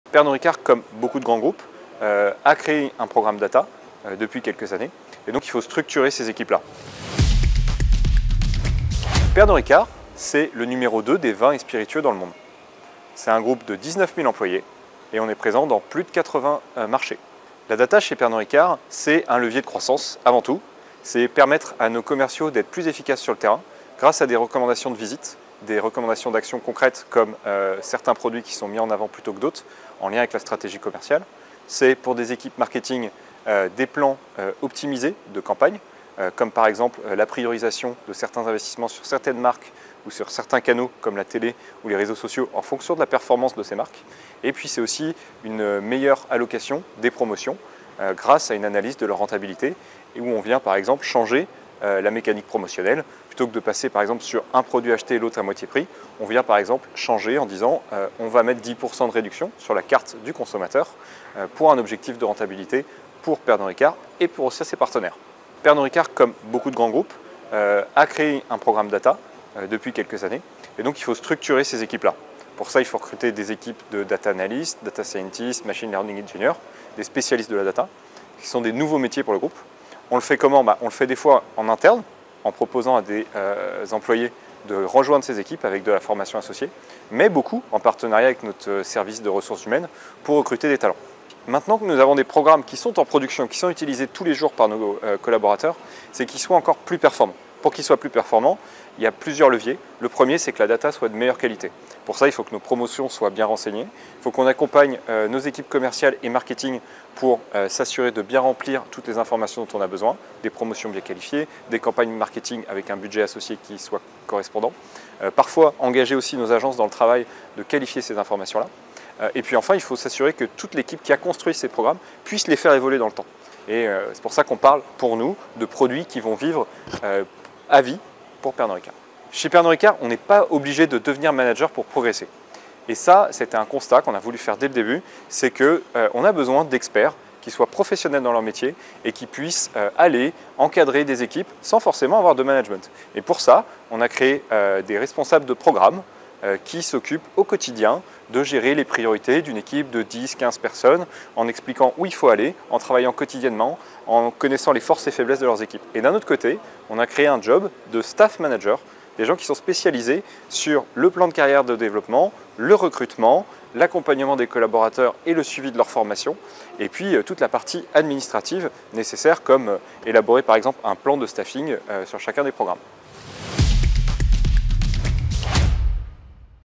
David Lepicier (Pernod Ricard) : « la data est un levier de croissance »
David Lepicier, global data design & analytics de Pernod Ricard, a été interviewé en vidéo l’occasion de sa venue à Républik Data, les 13 et 14 juin 2023 à Deauville.